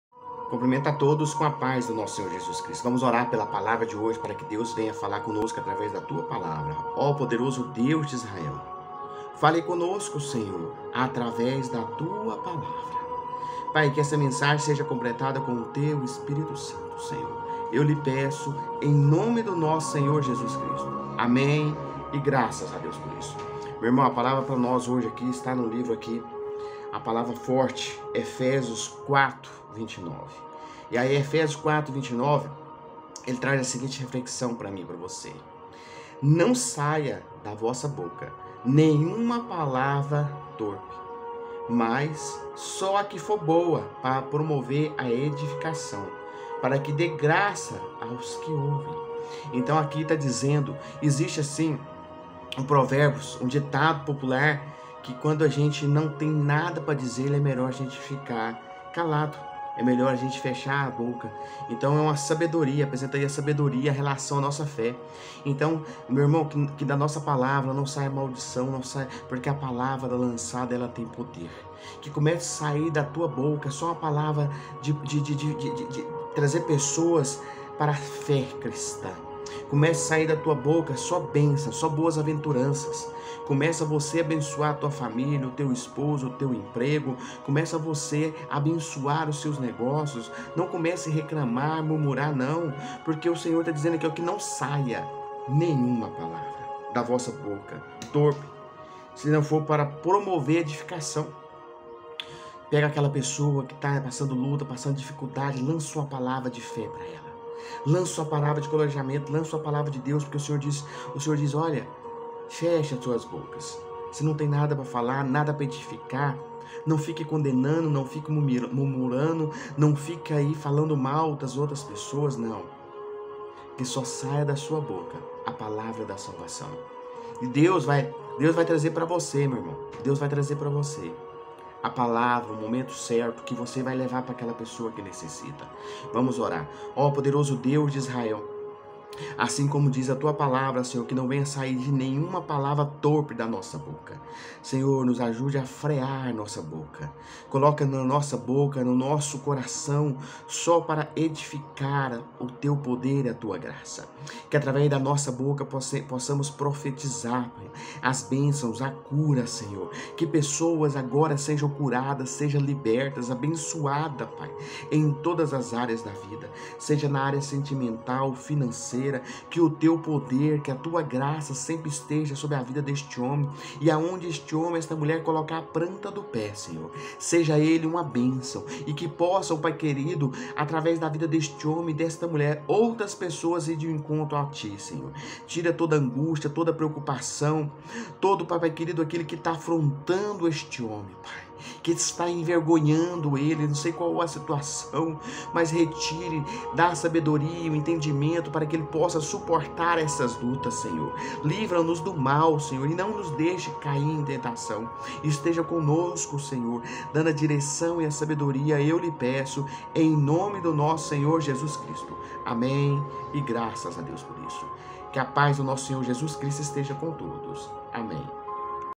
Não Saia da Vossa Boca Nenhuma Palavra Torpe - Efésios 4:29 ARA | Pr. Rodrigo Morais
Não saia da vossa boca nenhuma palavra torpe, e sim unicamente a que for boa para edificação, conforme a necessidade, e, assim, transmita graça aos que ouvem.

- Efésios 4:29 ARA

AG: 1626
CC: 5025-8
OP: 001